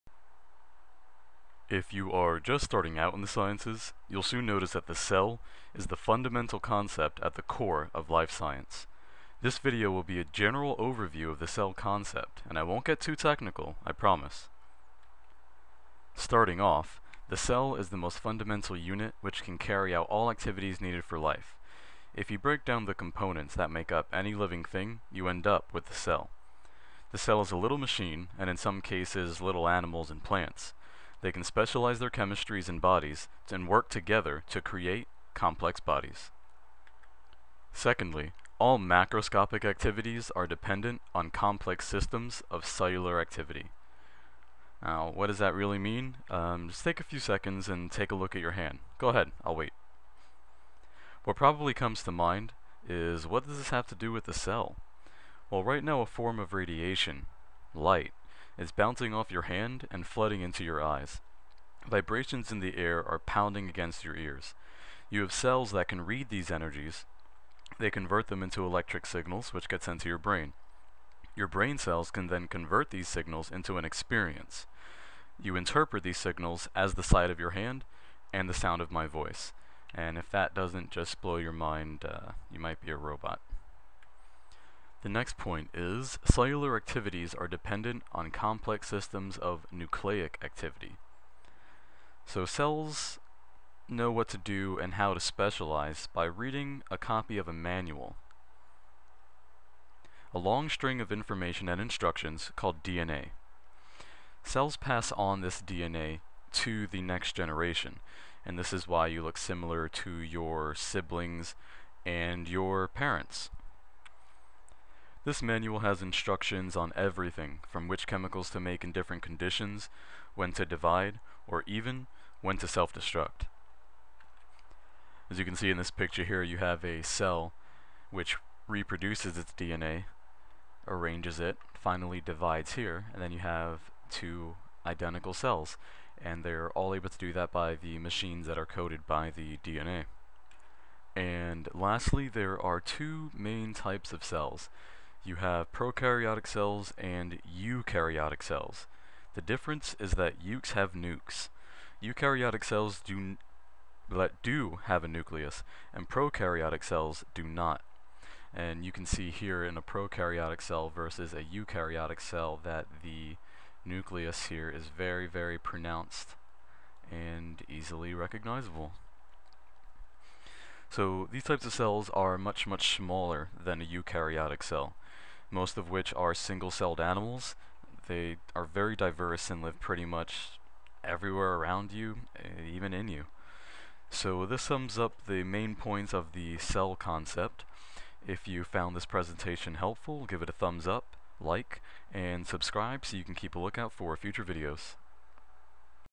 Biology Tutorial:Core Concept: The cell
A quick and dirty overview of the concept of the cell.